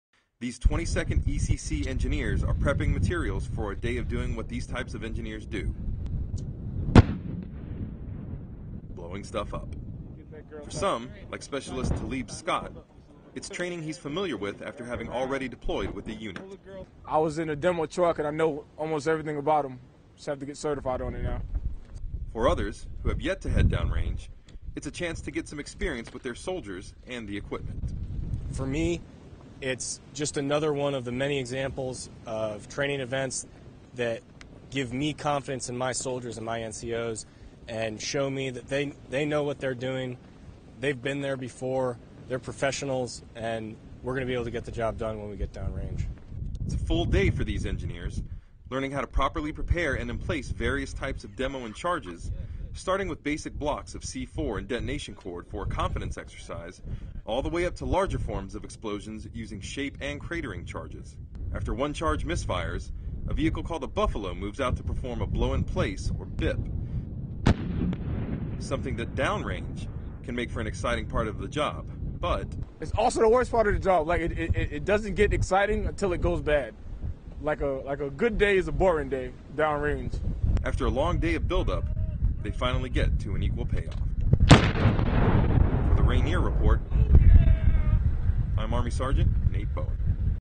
Engineers Train on Demolitions
Engineers from the 22nd Engineer Clearing Company Engineers spend a day at a demolition range. They begin with smaller charges and work their way up throughout the day to detonate shape and cratering charges. The training is in preparation for their upcoming deployment to Afghanistan later this year. Produced by Staff Sgt. Nathan Bowen